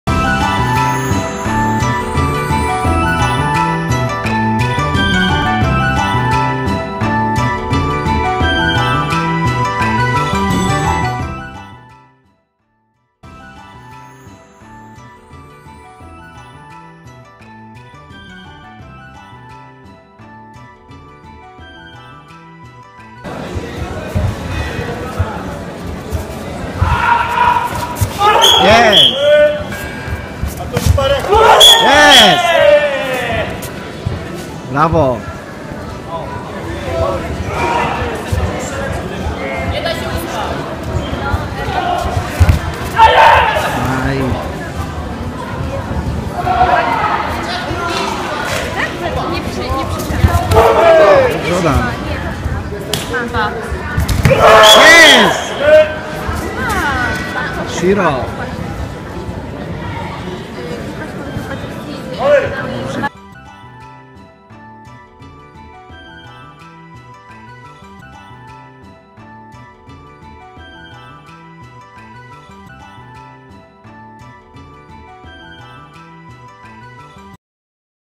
Półfinał Mistrzostw Europy Karate Fudokan
Oto najlepsze fragmenty mojej półfinałowej walki z  Mistrzostw Europy Karate Fudokan, Berlin 2014r.
Oj działo się :) zdobyłem wtedy 4 medale w tym złoto w kumite w swojej wadze i złoto w kata kobudo.